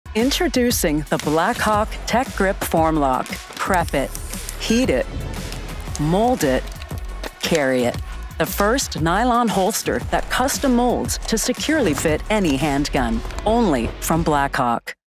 TecGrip FormLok - Custom moldable nylon holster
Custom-fit and ready to carry in just four steps. Achieve passive retention and make your mold with the all-new TecGrip FormLok holster.

If you have a handgun and are having trouble finding a holster that fits it, the TecGrip FormLok moldable IWB holster is the answer.

This technology-packed holster system begins with an outer layer of tacky material that clings to the body and fabric for secure all-day carry without needing a belt clip.

FormLok consists of a material called Memory-Lok. This Memory-Lok material is a thermoformable plastic that allows the user to heat up and mold the holster around their handgun, offering passive retention.

Every TecGrip FormLok is ambidextrous with a low-profile shirt guard for added comfort.